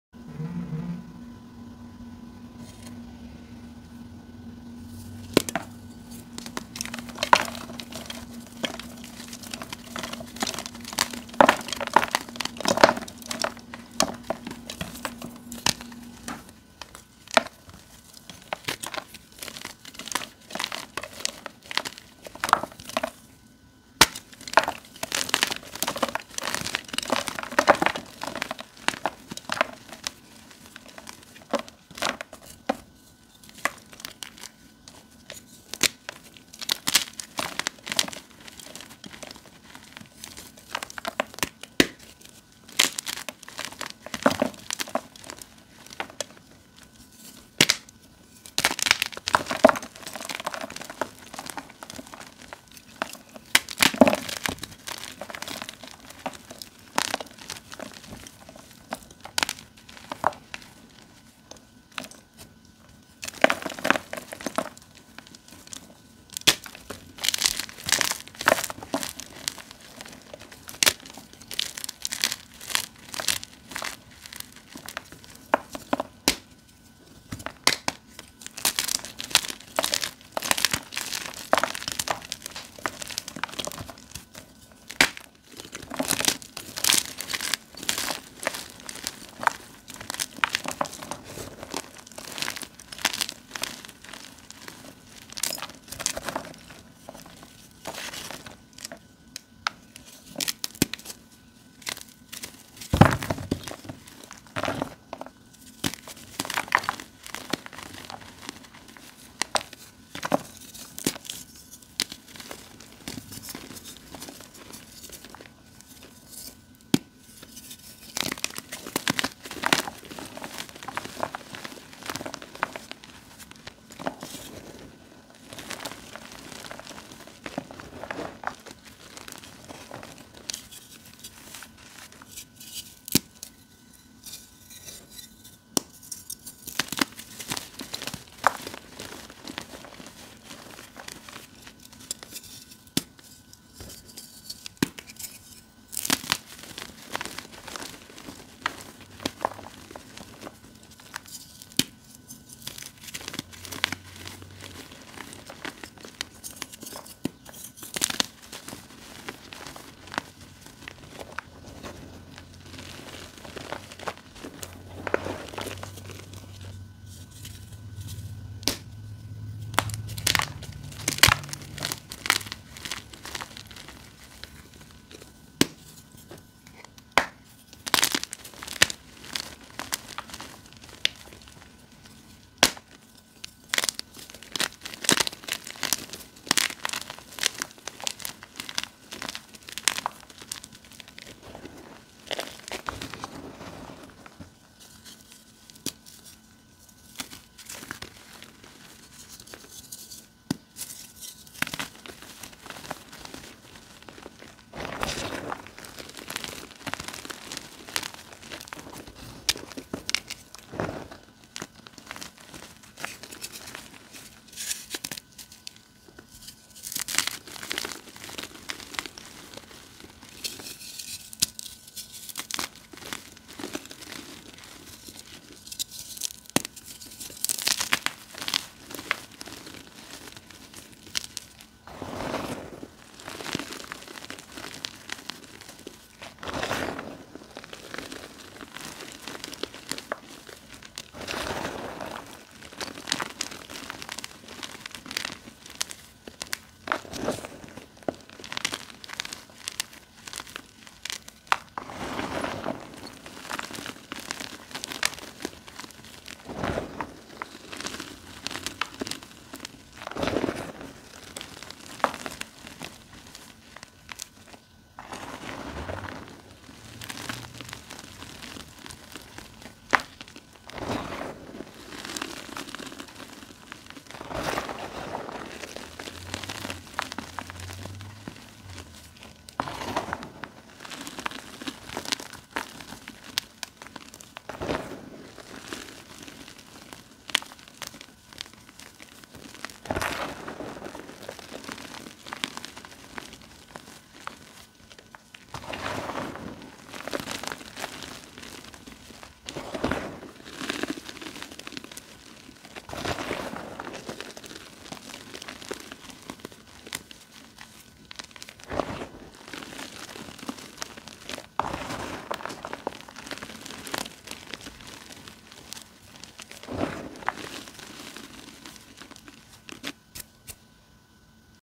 Not so Plain; Jane 😶‍🌫️ | Oddly Satisfying
Hey, everyone!

So this was my first time "undying" dyed blocks.  I remember seeing ‎@unplainjaneasmr  when she did the undyed blocks, and it was completely mind-bending and cool. I didn't have the desire to try it, mainly because I typically just do my own thing.  But as time progressed, I saw other creators who were also doing it, and I pro.osed myself that I'd try it at some point.

It was actually pretty cool to see the color once I cracked the "white" blocks open. I'm not sure if I'll be rushing to do this again anytime soon, but it was definitely a very unique and creative experience.

I hope you guys enjoy the crush!

🤍 Kim